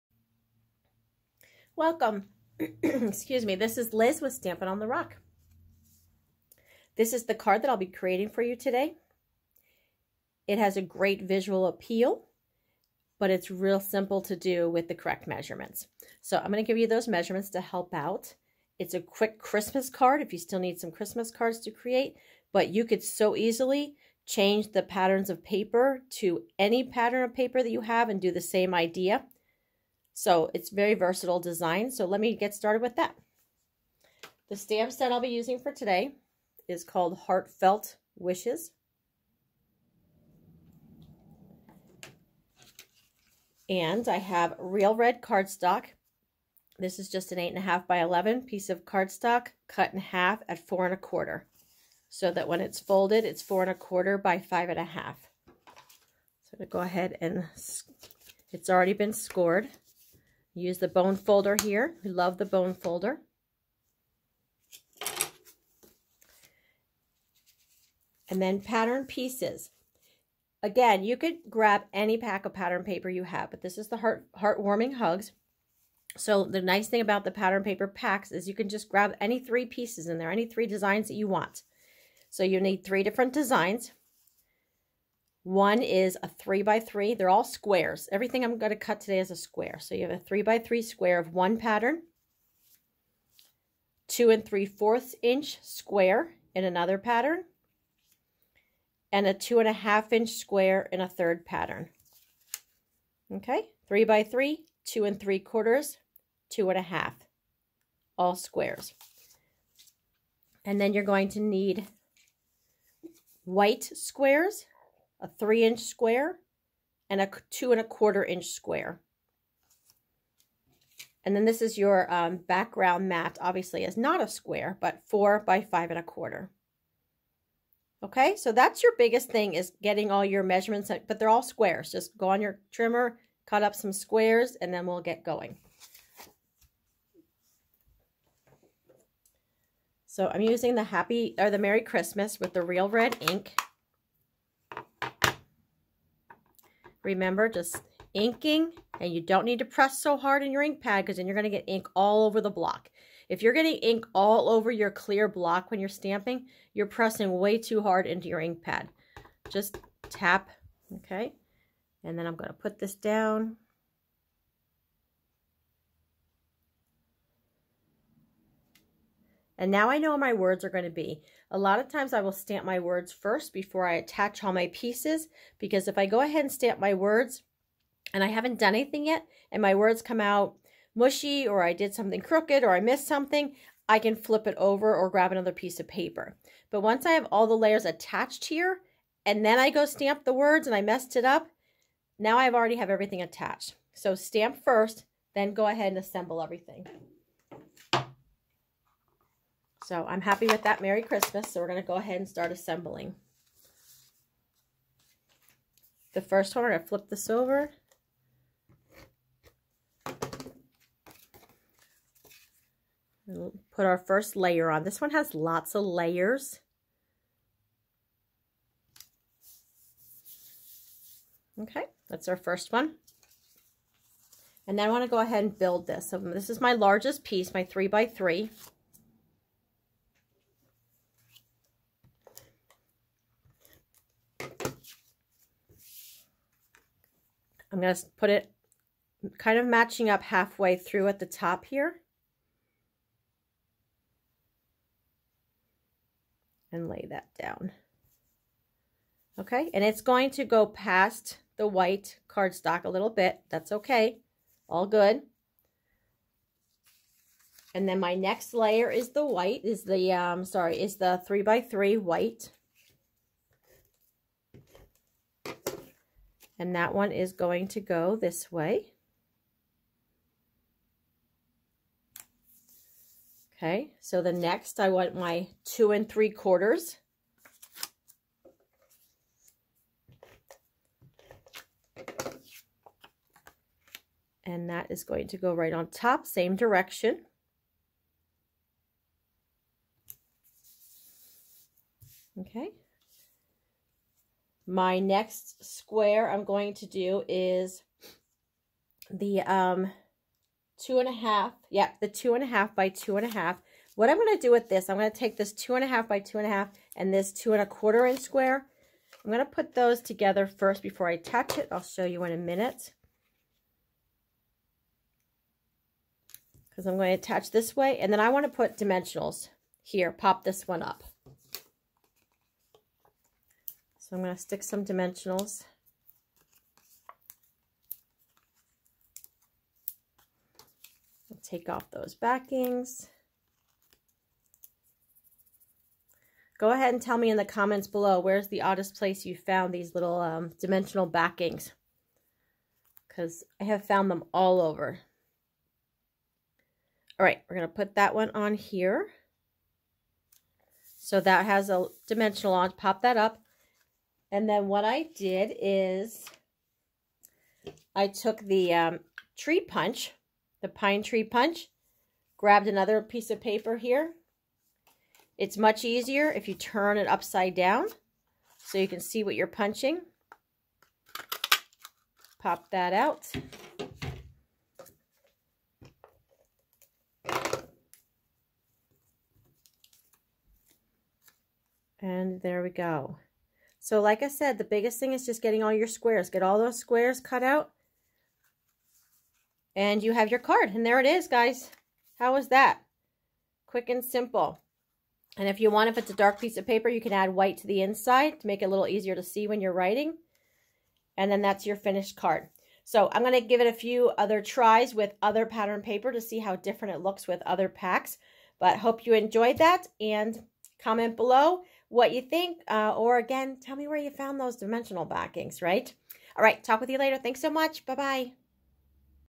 Lunchtime with Liz- Layered Squares
Simple and sweet, multi layered card using squares.